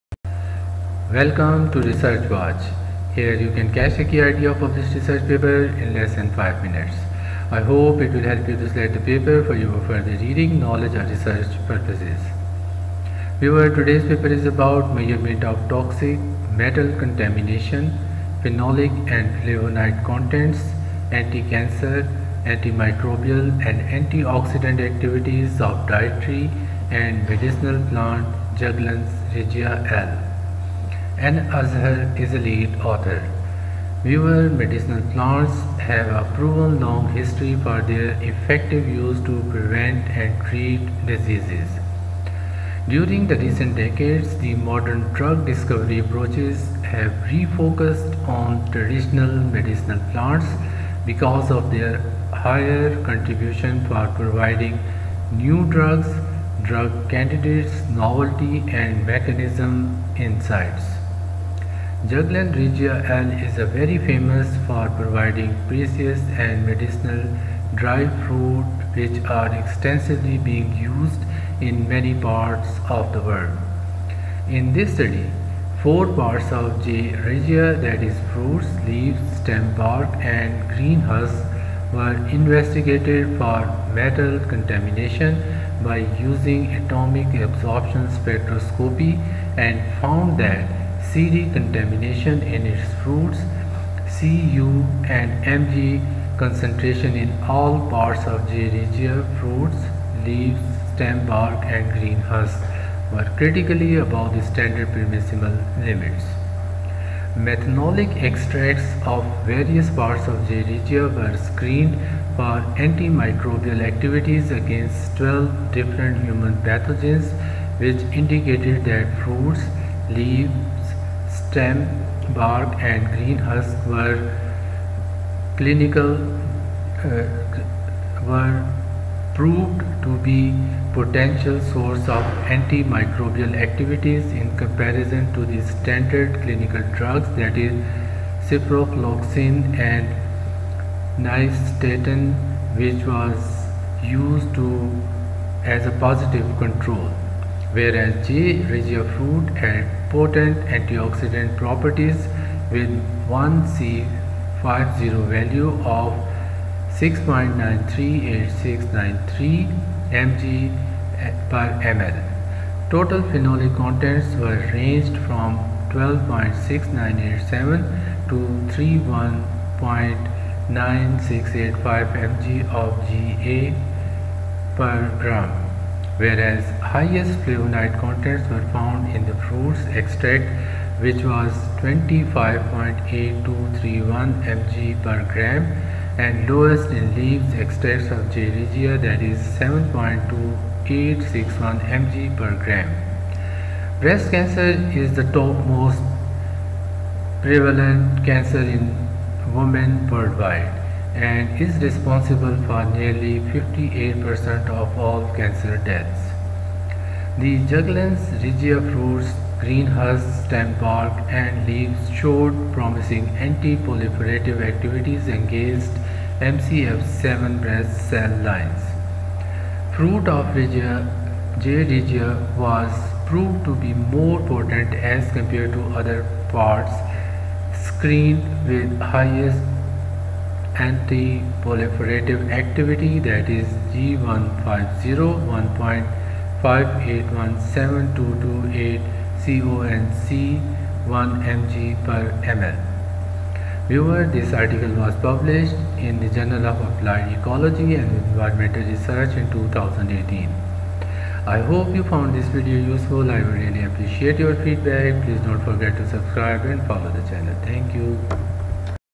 MEASUREMENT OF TOXIC METAL CONTAMINATION, PHENOLIC ...
MEASUREMENT OF TOXIC METAL CONTAMINATION, PHENOLIC AND FLAVONOID   CONTENTS, ANTICANCER, ANTIMICROBIAL AND ANTIOXIDANT ACTIVITIES OF   DIETARY AND MEDICINAL PLANT JUGLANS REGIA L.